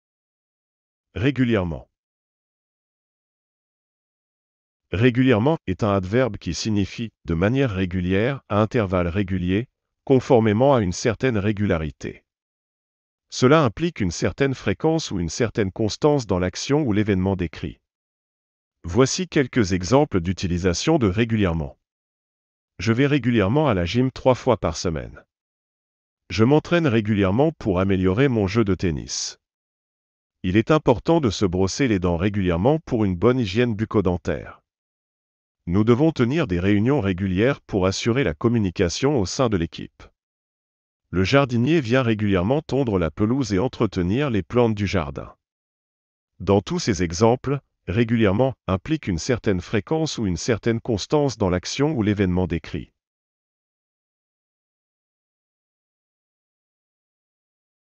Régulièrement
"Régulièrement" est un adverbe qui signifie "de manière régulière, à intervalles réguliers, conformément à une certaine régularité". Cela implique une certaine fréquence ou une certaine constance dans l'action ou l'événement décrit.
Voici quelques exemples d'utilisation de "régulièrement" :
- "Je vais régulièrement à la gym trois fois par semaine."
- "Je m'entraîne régulièrement pour améliorer mon jeu de tennis."
- "Il est important de se brosser les dents régulièrement pour une bonne hygiène bucco-dentaire."
- "Nous devons tenir des réunions régulières pour assurer la communication au sein de l'équipe."
- "Le jardinier vient régulièrement tondre la pelouse et entretenir les plantes du jardin."
Dans tous ces exemples, "régulièrement" implique une certaine fréquence ou une certaine constance dans l'action ou l'événement décrit.